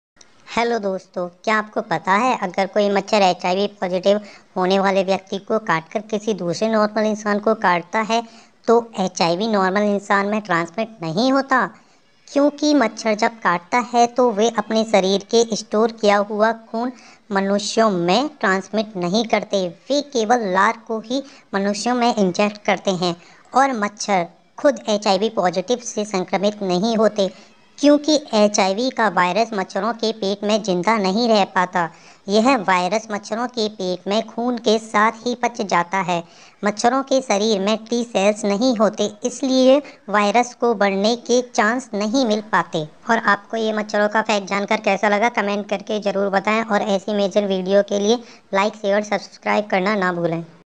Kya aapko pta h machhar🦟 ke katne se HIV nhi hota BackToBasics by dussii
Kya aapko pta hai machhar ke katne se HIV nhi hota.

Fact videos